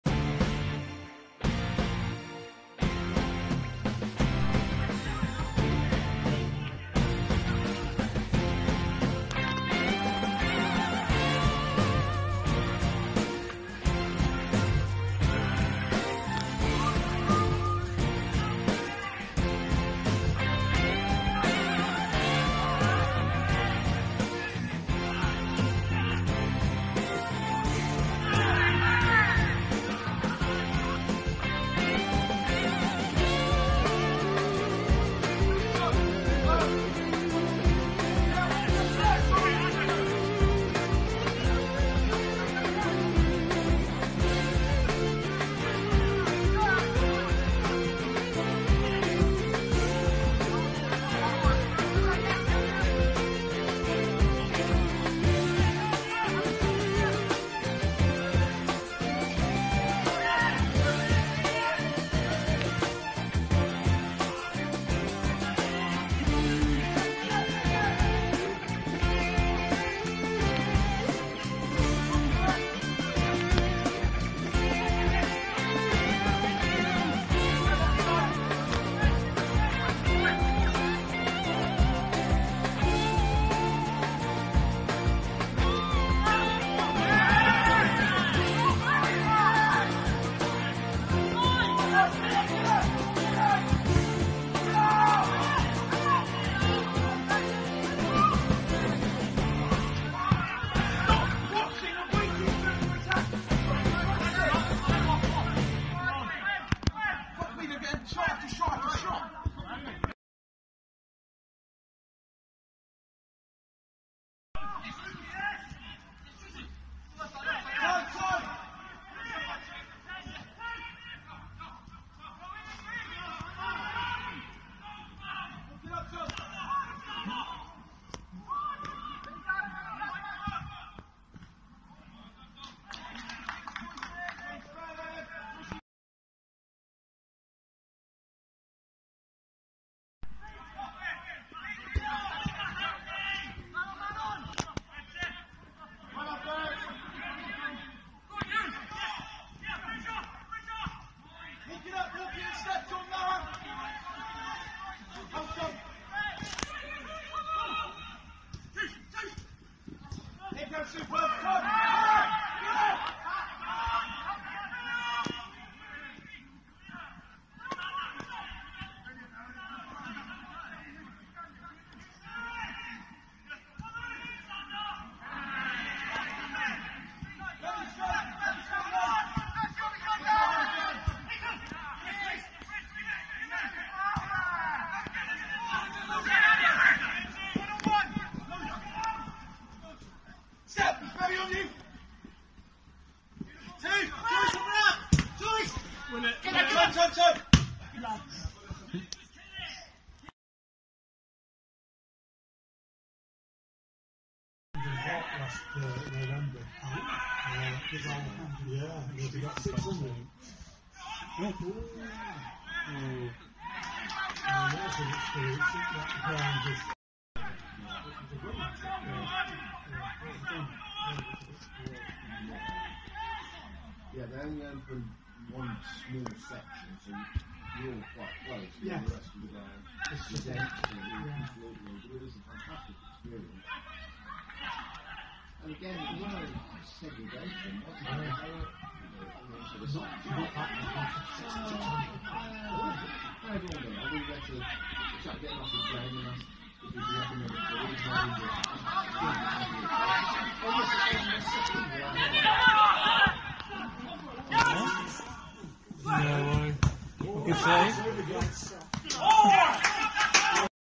Newton Abbot Spur FC vs Torpoint FC
A game on cold Wednesday night which ended 1-0 to spurs